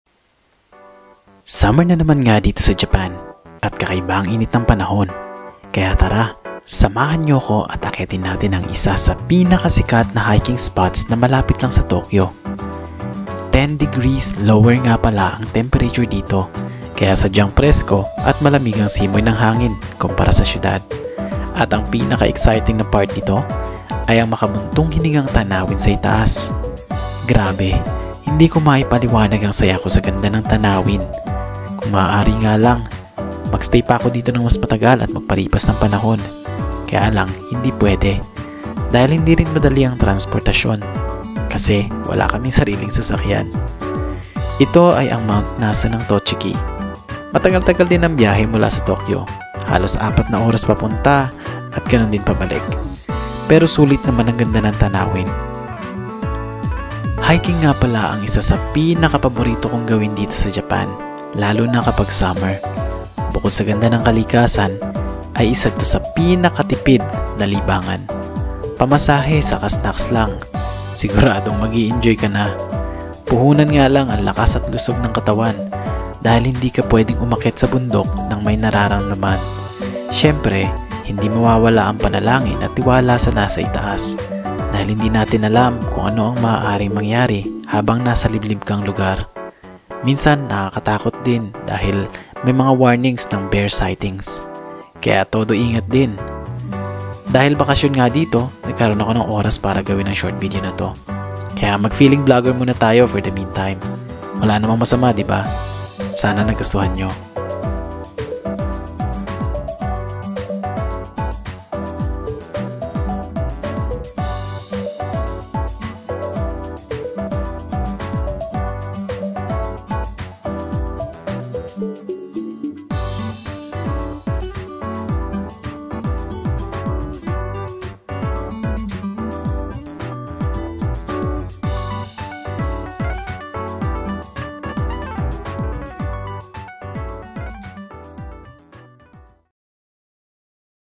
Hiking Mt. Nasu Tochigi, Japan
sharing my hike at Mt. Nasu in Tochigi.